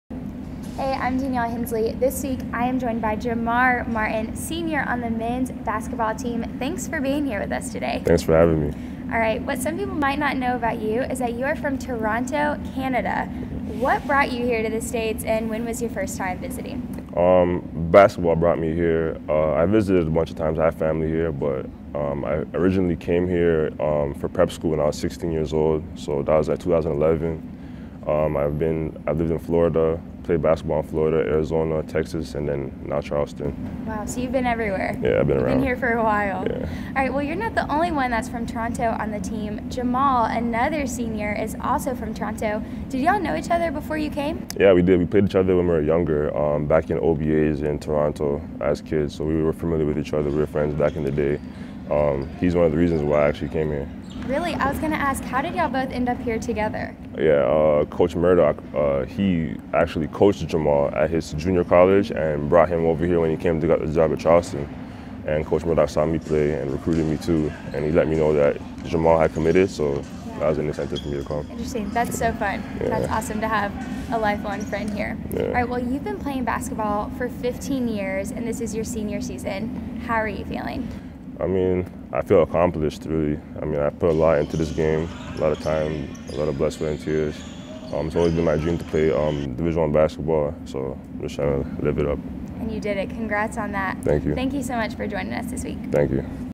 Friday Feature - Jamar Martin
Danielle Hensley sits down with Charleston Southern senior forward Jamar Martin this week's Friday Feature. Martin and the Bucs are in the midst of a two-week break from game action before traveling to South Carolina State on Tuesday, Dec. 12.